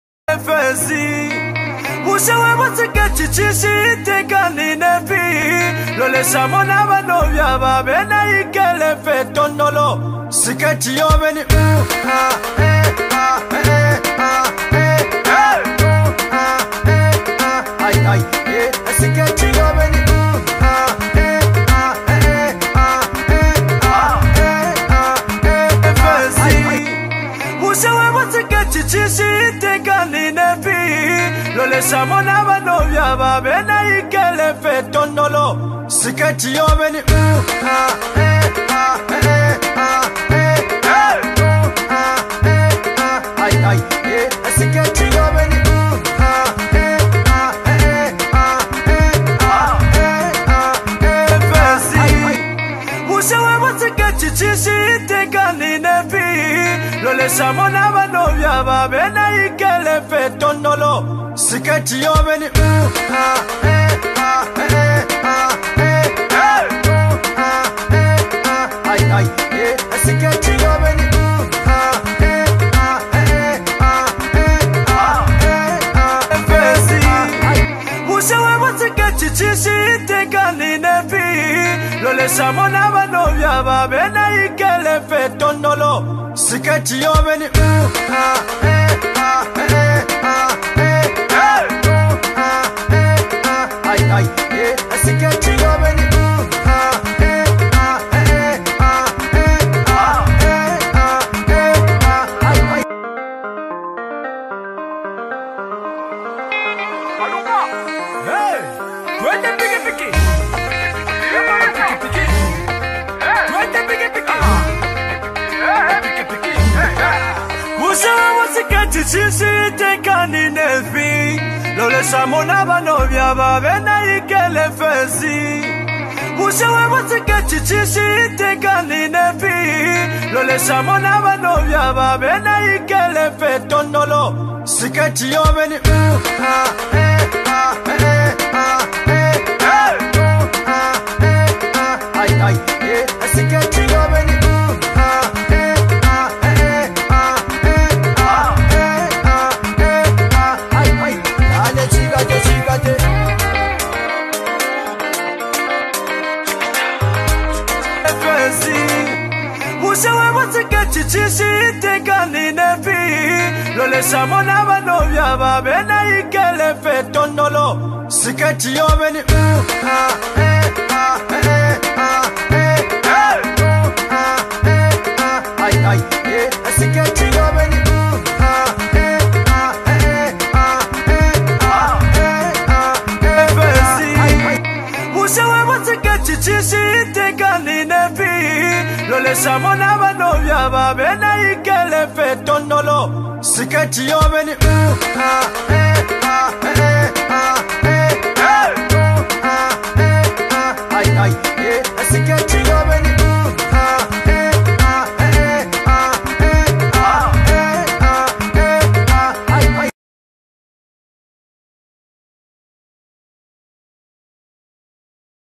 BEST TIK TOK AFRICA WAIST CHALLENG COMPILATION piki piki challenge
Piki Piki Challenge, with Africa's best waist dancers. Enjoy!